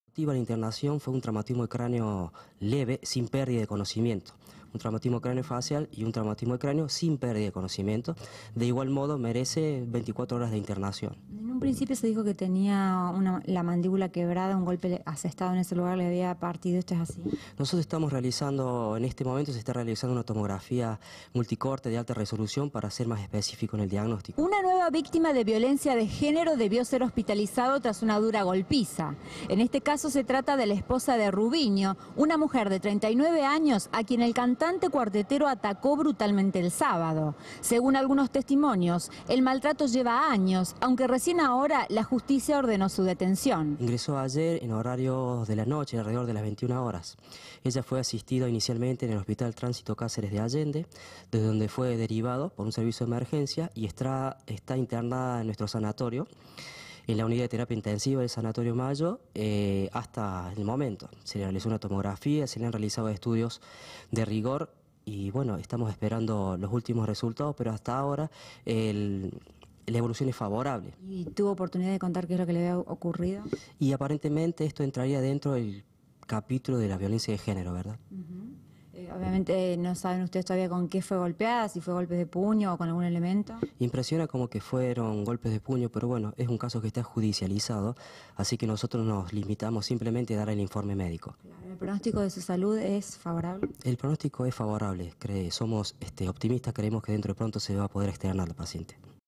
Violencia de género: detienen a Rubinho por golpear a su esposa
El cantante fue reducido por los vecinos. La mujer sufrió fracturas y traumatismos. Se encuentra internada en un sanatorio.